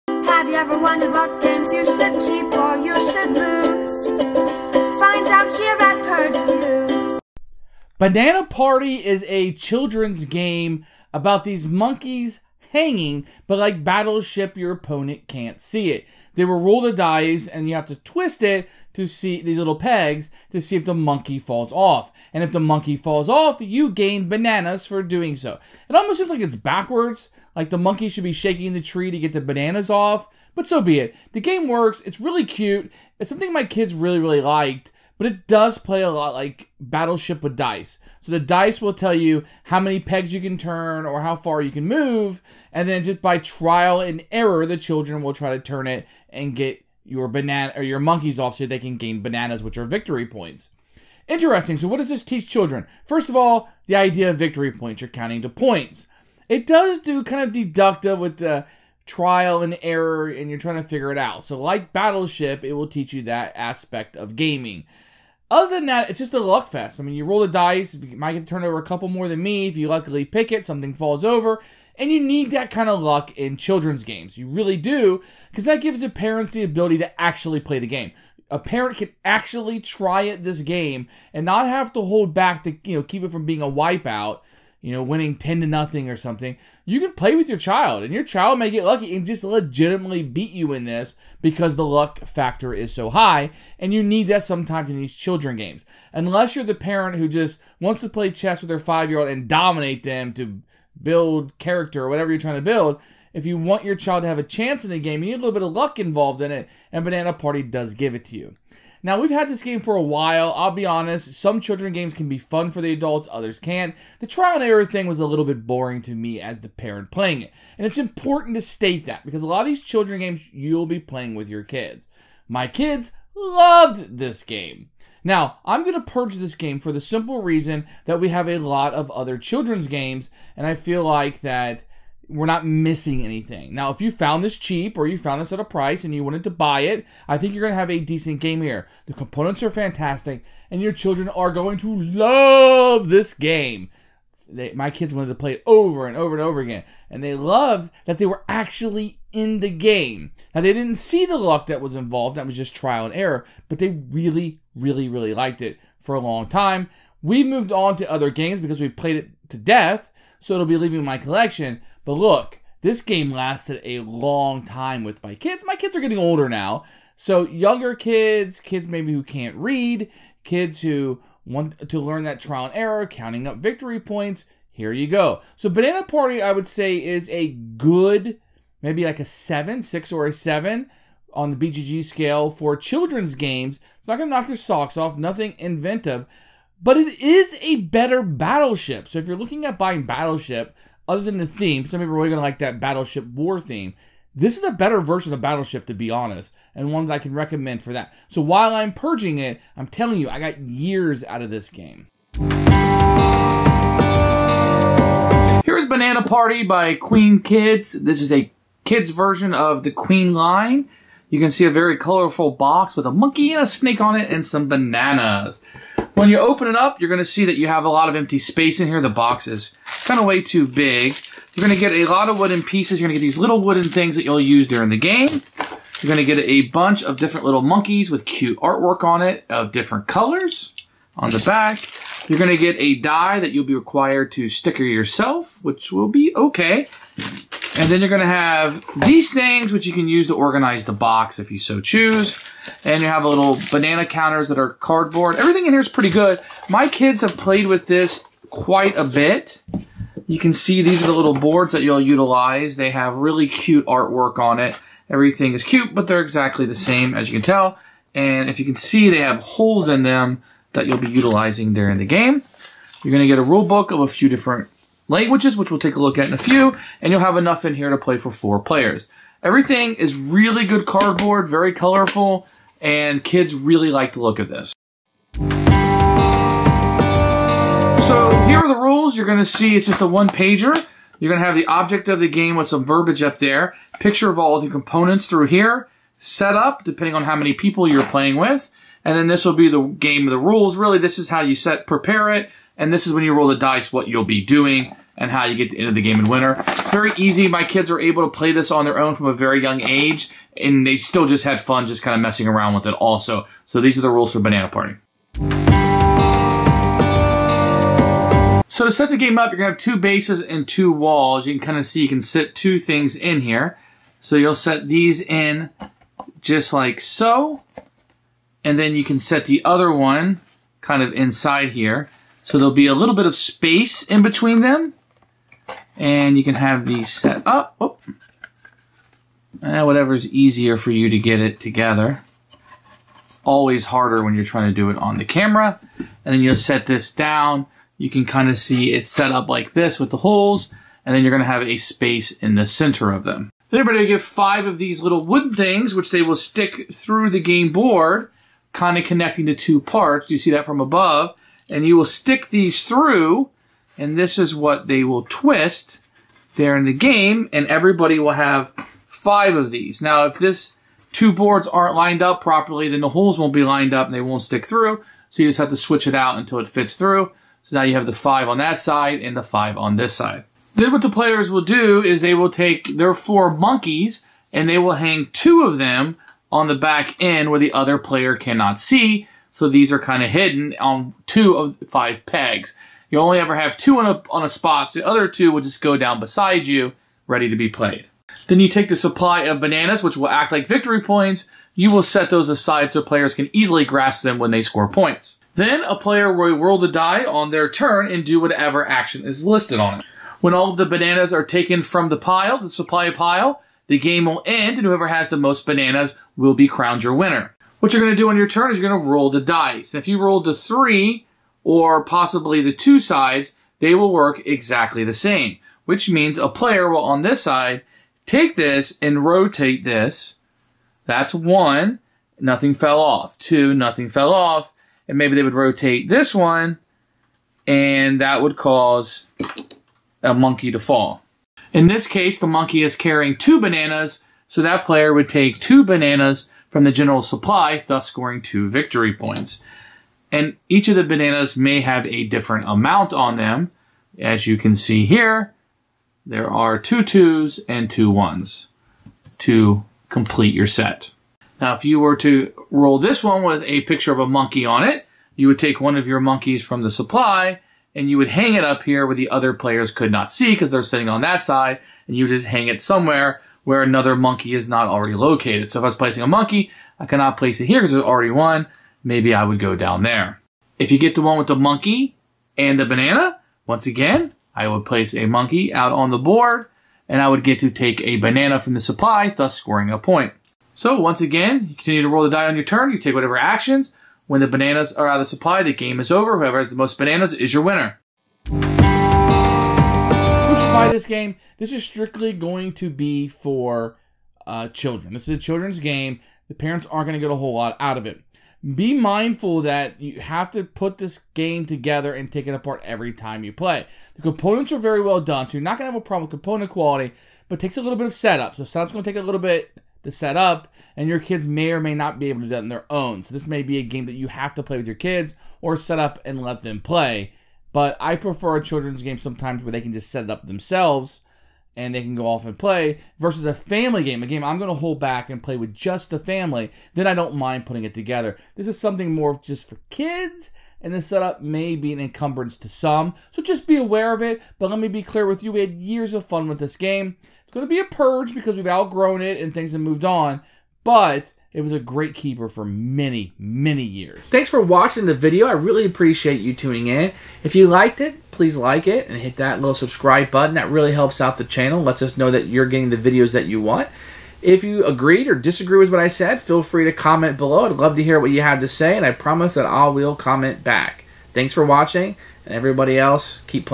Banana Party
This is a board game review of Banana Party. Banana Party is a children's game published by Queen Games. This game stars monkeys trying to get bananas.  This takes the classic game of Battleship  and turns it on its head.